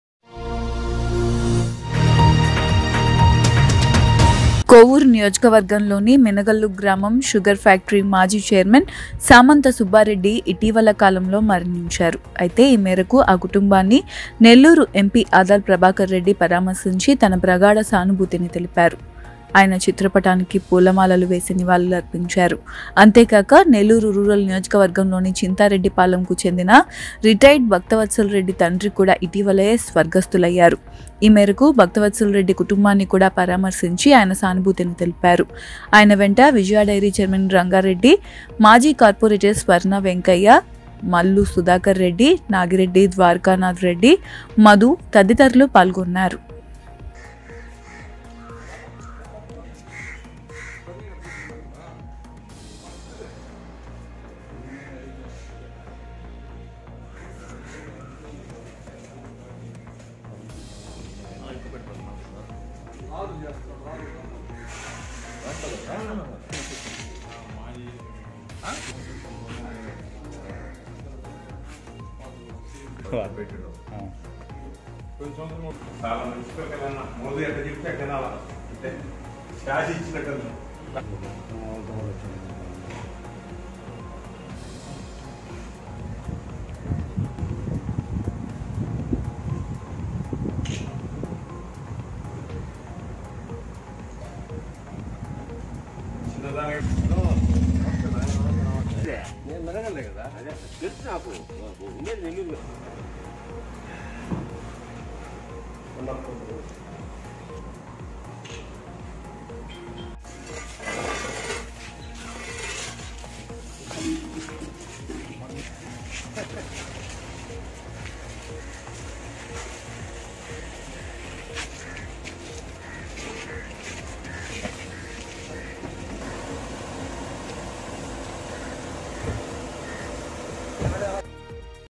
SUGAR FACTORY X CHAIRMEN DEAD , MP ADALA PRBHAKAR REDDY CONDOLE/BUCHIREDDYPALEM/NELLORE/ CHAMUNDI TV
కొవ్వూరు నియోజకవర్గం లోని మినగల్లు గ్రామ షుగర్ ఫ్యాక్టరీ మాజీ చైర్మన్ సామంత సుబ్బారెడ్డి ఇటీవల కాలంలో మరణించారు. అయితే ఈ మేరకు ఆ కుటుంబాన్ని నెల్లూరు ఎంపీ ఆదాల ప్రభాకర్ రెడ్డి పరామర్శించి తన ప్రగాఢ సానుభూతిని తెలిపారు. ఆయన చిత్రపటానికి పూల మాల వేసి నివాళులర్పించారు. అంతేకాక నెల్లూరు రూరల్ నియోజకవర్గం లోని చింతా రెడ్డిపాలెం కు చెందిన రిటైర్డ్ భక్తవత్సల రెడ్డి తండ్రి కూడా ఇటీవలే స్వర్గస్తులయ్యారు. ఈ మేరకు భక్తవత్సల రెడ్డి కుటుంబాన్ని కూడా పరామర్శించి ఆయన సానుభూతిని తెలిపారు. ఆయన వెంట విజయ డైరీ చైర్మన్ రంగారెడ్డి, మాజీ కార్పొరేటర్ స్వర్ణ వెంకయ్య, మల్లు సుధాకర్ రెడ్డి, నాగిరెడ్డి ద్వారకానాథ్ రెడ్డి, మధు తదితరులు ఉన్నారు.